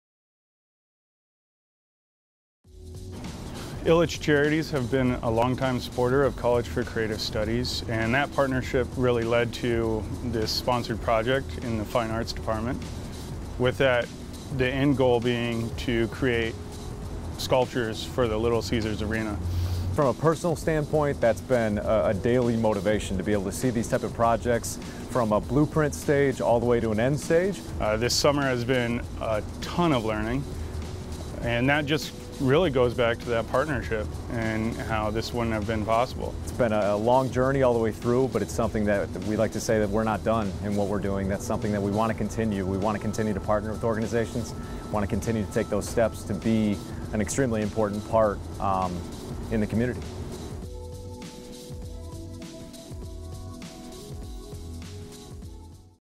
Association of Fundraising Professionals-GDC - 2018 Outstanding Foundation Award
Ilitch Charities is the latest recipient of the Association of Fundraising Professionals Outstanding Foundation Award. Take a look at the project they worked with College for Creative Studies for the new Little Caesar's Arena.